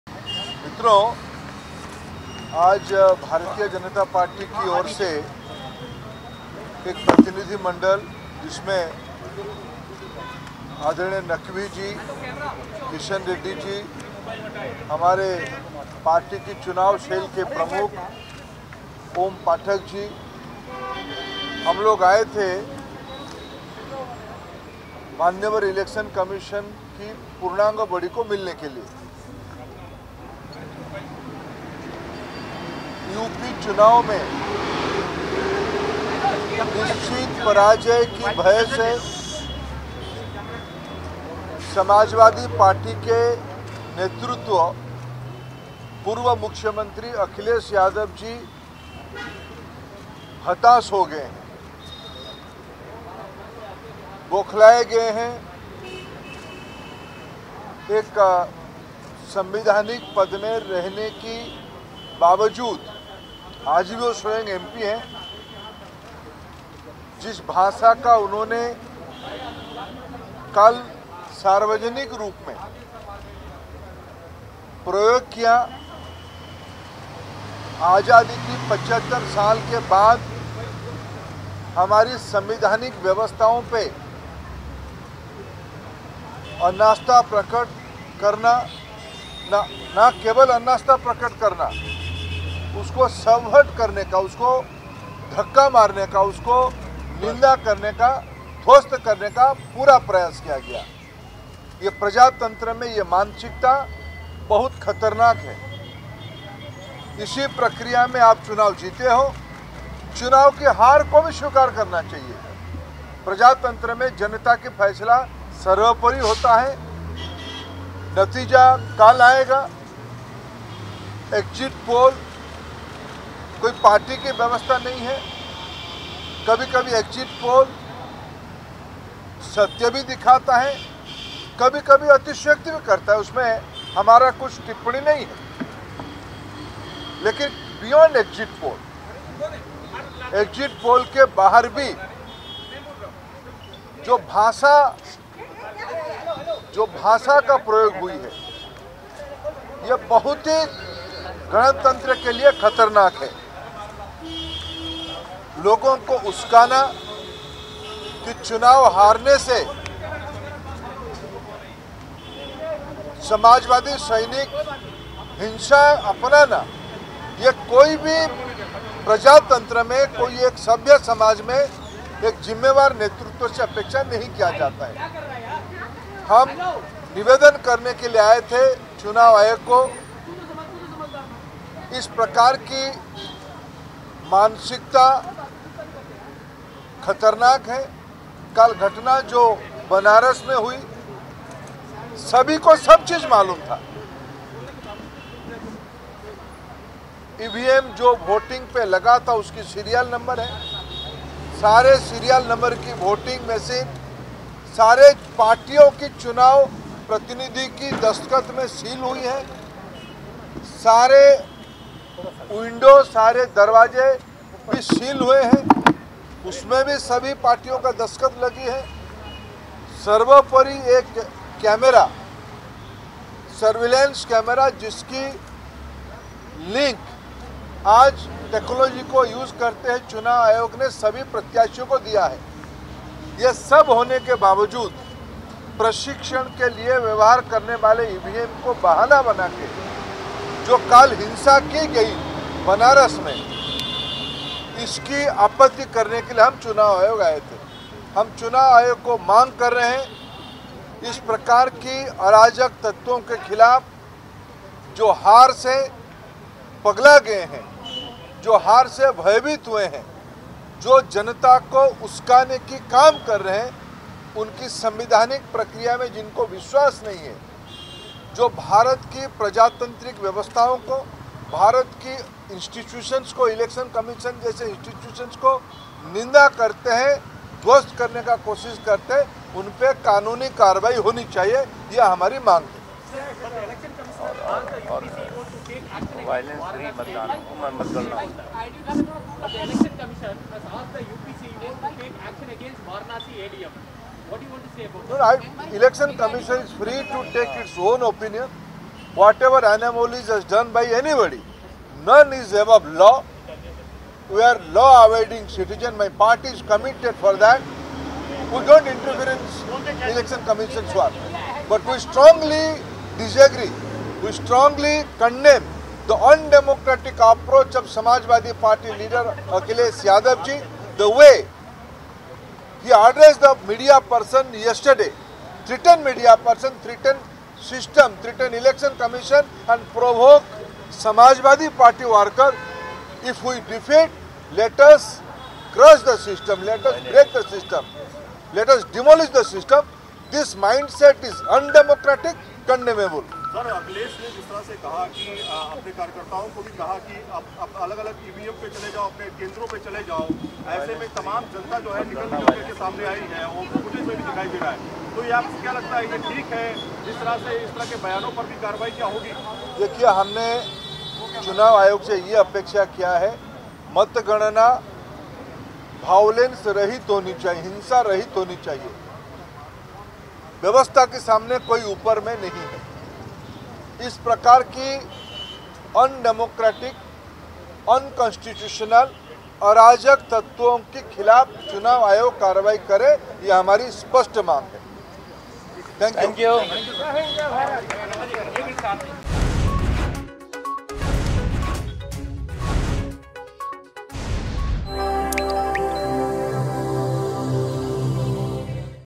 BJP's Dharmendra Pradhan has Lodged a Complaint with the Election Commission
BJP has lodged a complaint with the Election Commission. Samajwadi Party chief and former Uttar Pradesh chief minister Akhilesh Yadav has insulted the country's basic system. He also incited his party workers to violence. Union Minister and UP in-charge Dharmendra Pradhan met the Election Commission for legal action.